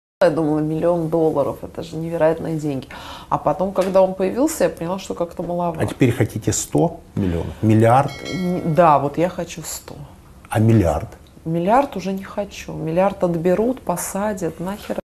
Сколько мне нужно денег? Ксения Собчак
На Интервью Ксения Собчак призналась сколько у нее денег и сколько она хочет.

Спасибо!